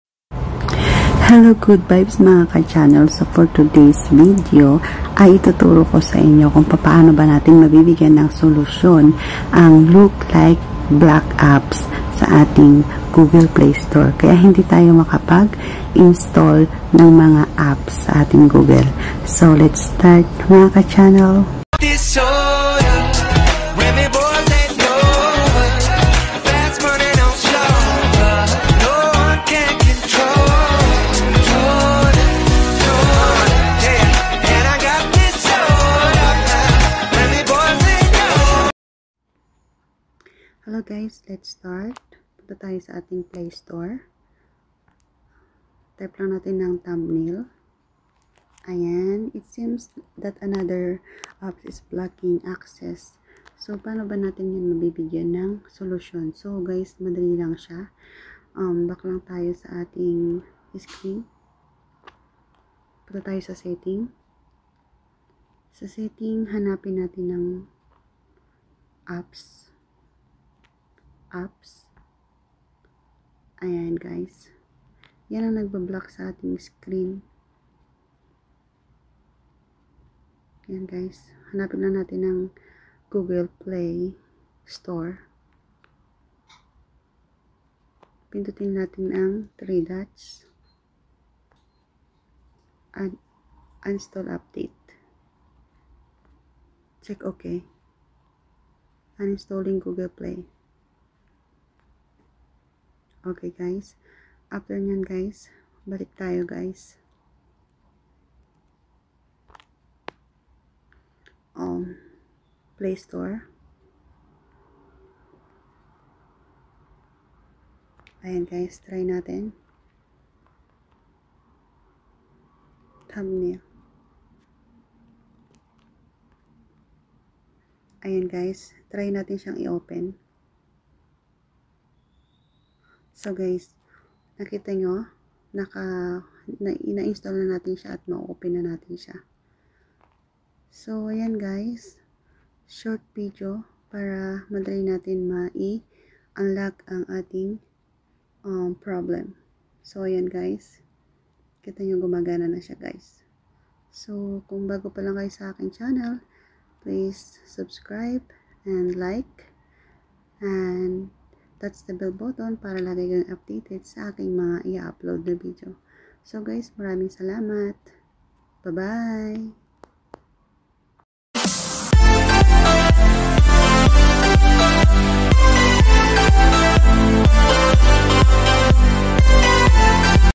EASY WAYS UNBLOCKED GOOGLE PLAYSTORE#FIX LOOK LIKE BLOCKING APPS.
BLOCKING ACCESS  PARIN SA GOOGLE PLAY KHIT D GUMAGAMIT NG SCREENRECORD Madaling paraan... MAY SOLUSYON JAN.